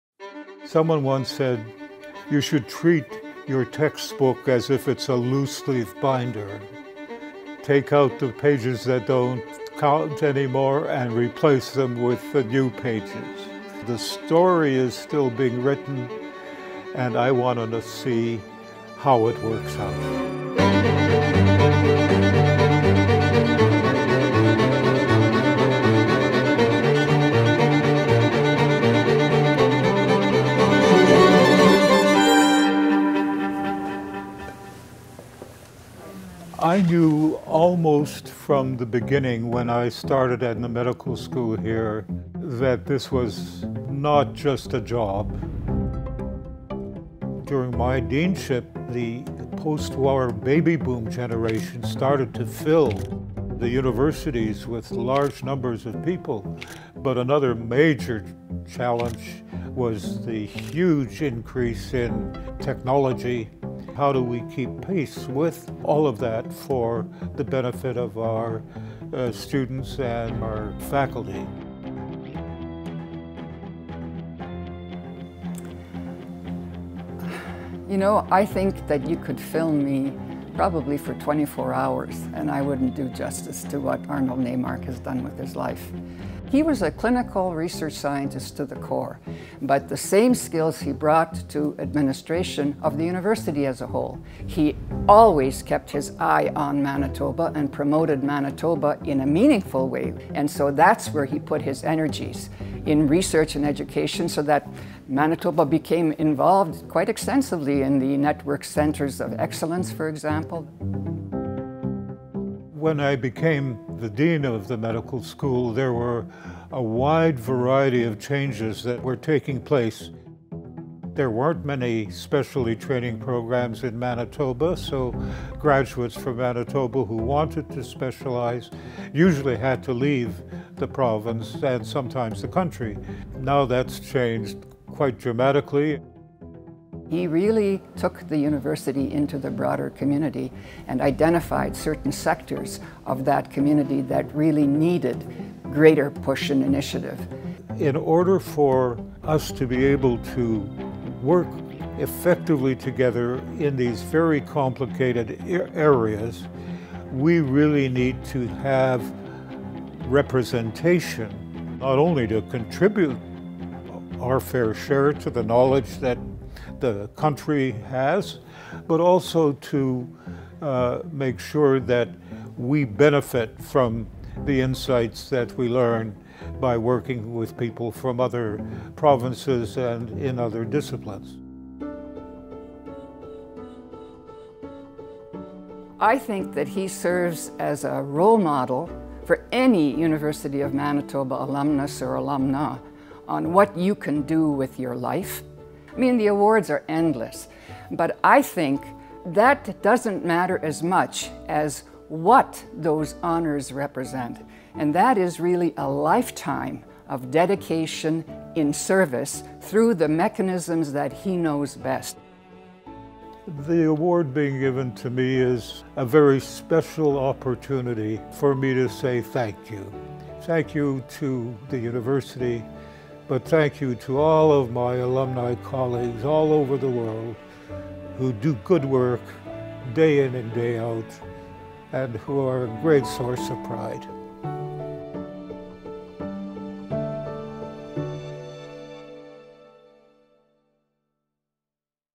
Distinguished Alumni Awards 2022 Lifetime Achievement Award
Celebrating Dr. Arnold Naimark [BSc(Med)/57, MD/57, MSc/60] at the University of Manitoba Distinguished Alumni Awards 2022 for Lifetime Achievement Award. For his lifelong commitment to promoting health care research funding and education in this province and at the University as former Dean of the Rady Faculty of Health Sciences, distinguished service as former UM president and vice-chancellor, and significant volunteer service with numerous organizations.